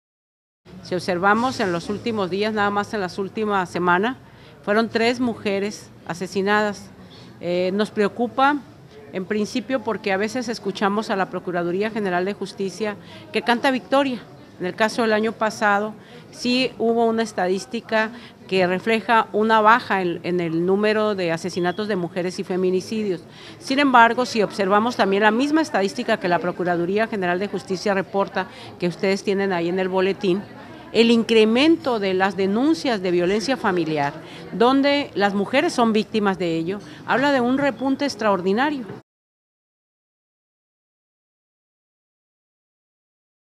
Colectivo de mujeres por sinaloa
Preocupa repunte de violencia contra las mujeres